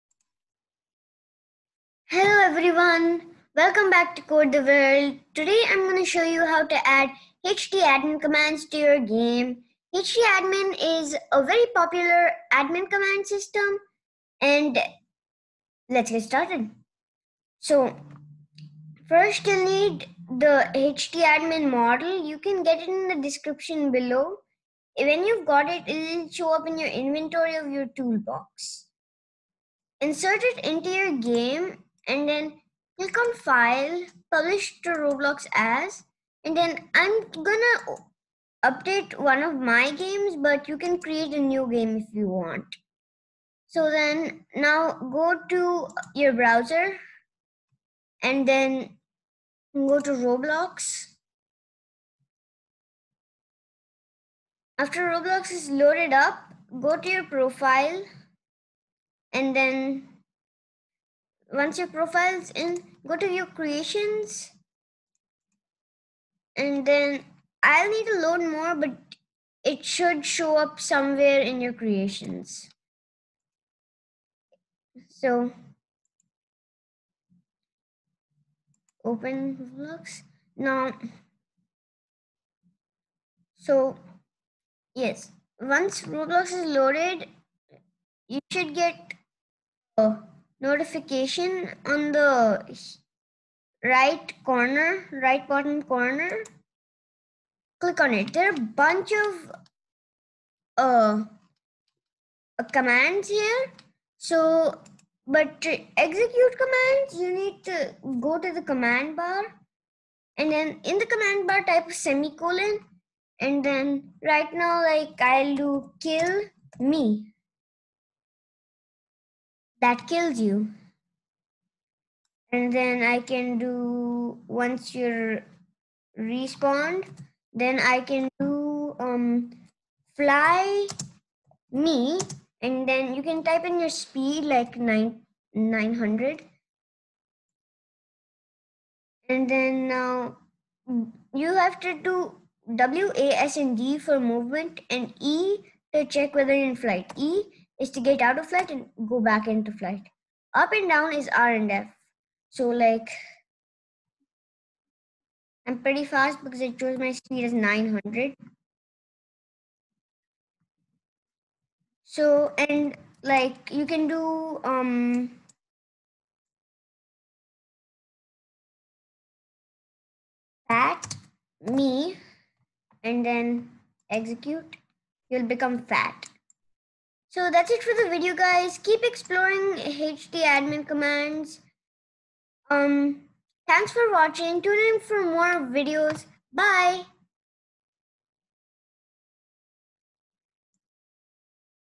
Easy tutorial to insert HD Admin Commands Into Your Roblox Game
In This Video I Will Walk You Through On How To Insert And Use HD Admin Command In Your Game.